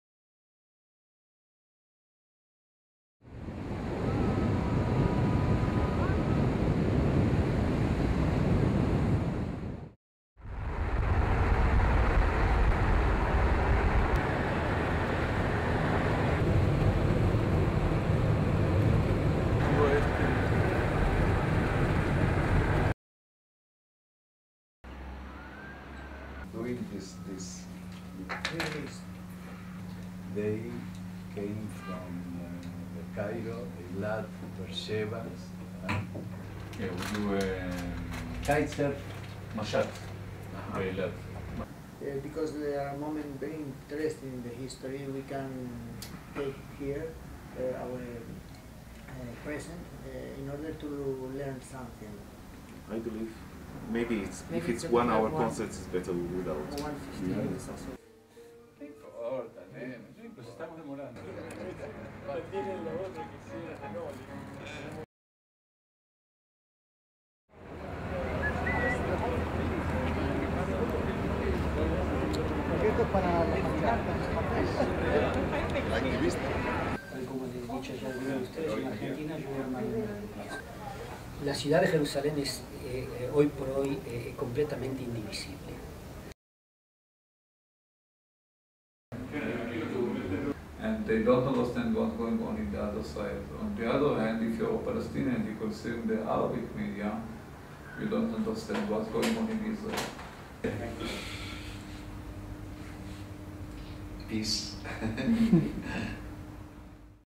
Meetings in Jerusalem - 12 october 2009
Meetings in Jerusalem - 12 october 2009 - Middle East World March for Peace and Noviolence